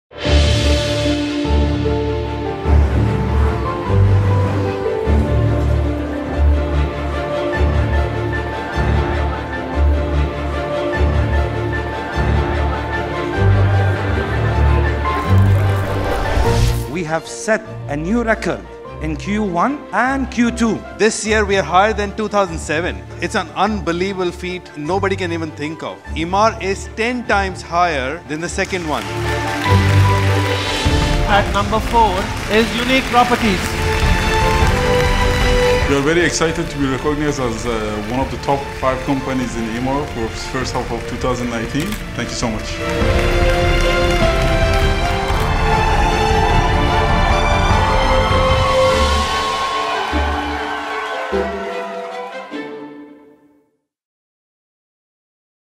Emaar Half-Yearly Broker Awards 2019 - Unique Properties
Highlights from Emaar’s Half-Yearly Broker Awards event where we were recognized as Emaar's Top Broker for the first two quarters of 2019.

We are truly proud of this marvelous achievement and would like to give our greatest gratitude and appreciation towards our trusted clients and our amazing channel partner Emaar in particular for having confidence in our work and we look forward to our next collaboration and continues growth for both our companies.